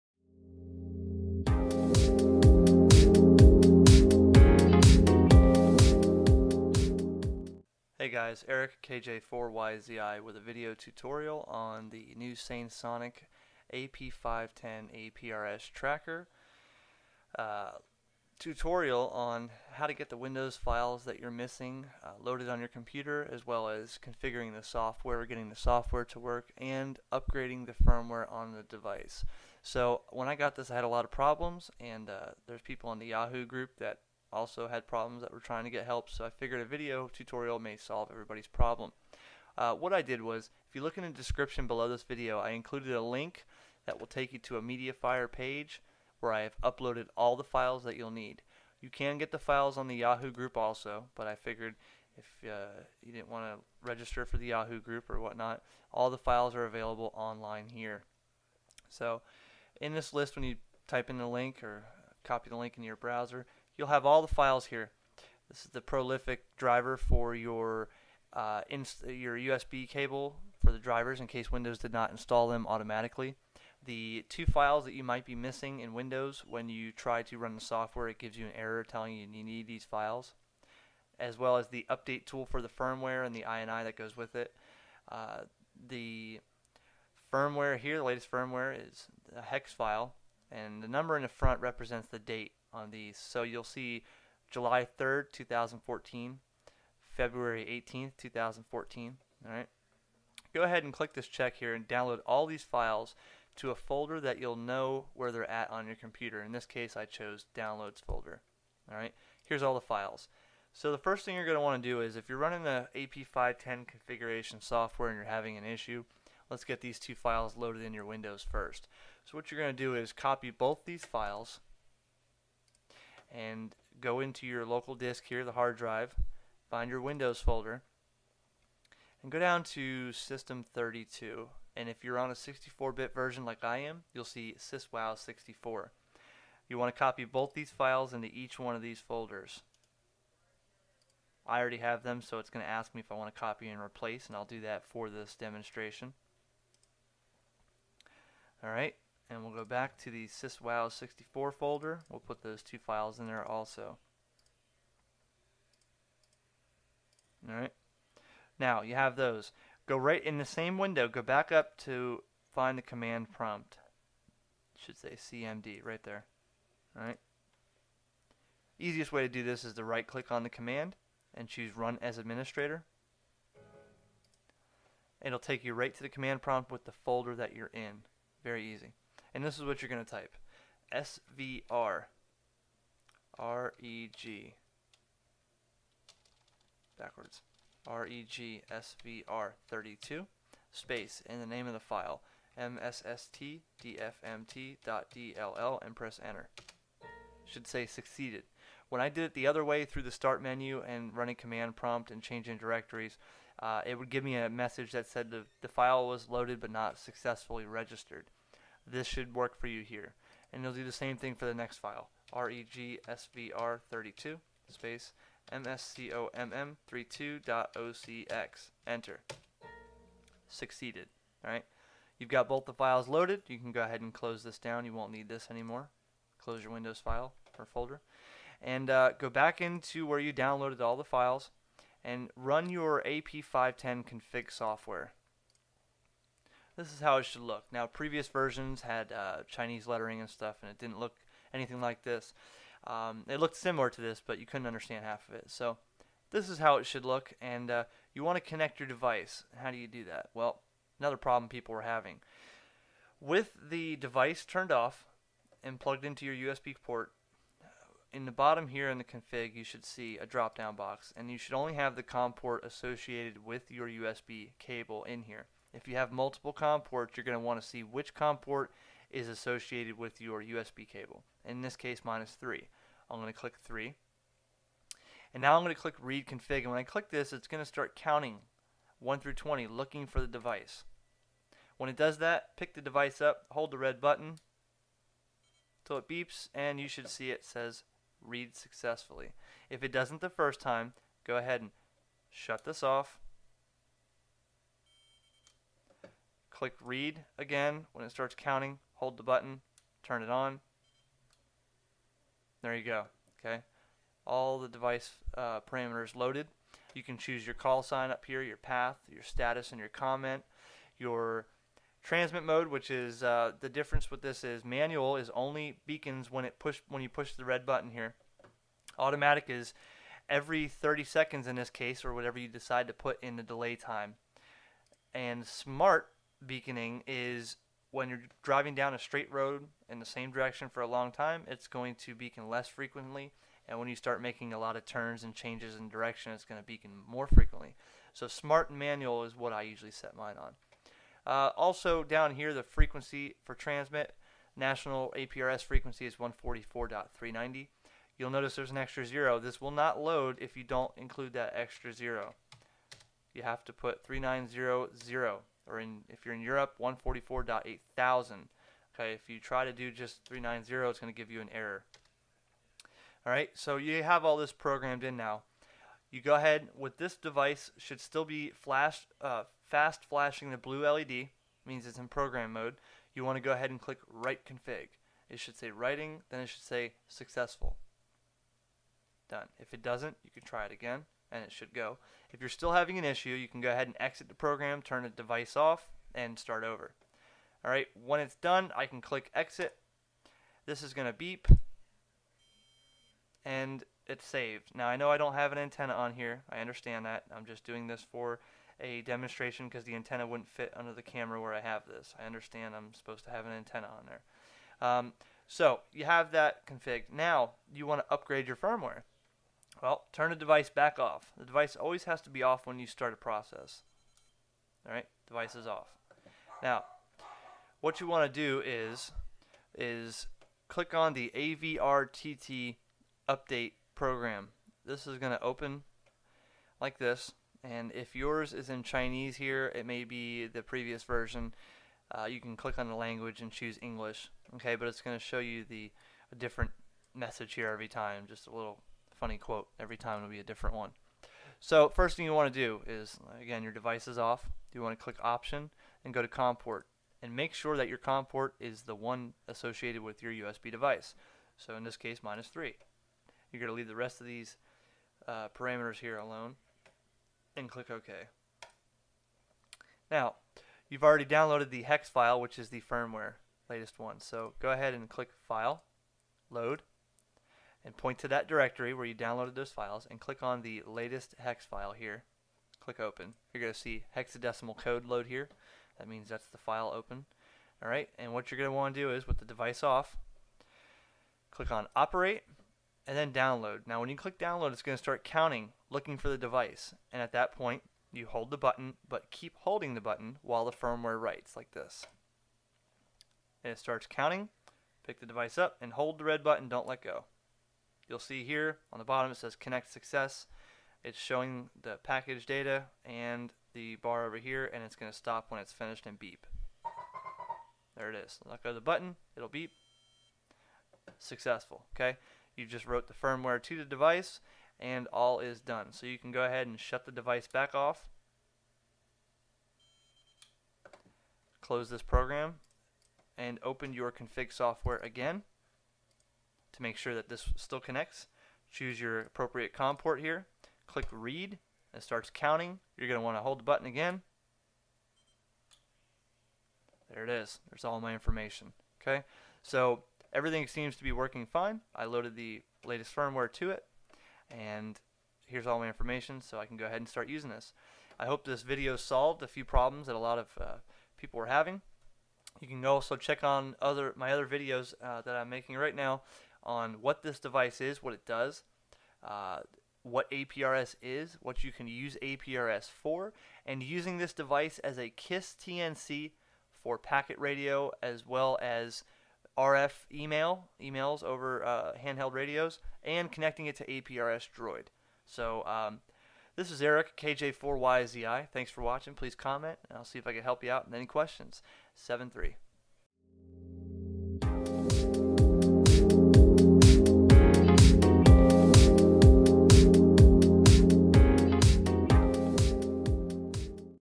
AP510 APRS tracker configure software and firmware
Link to files to download

Having trouble with your new AP510 aprs tracker by sainsonic? cant connect the device to your software? firmware wont download? this video should help..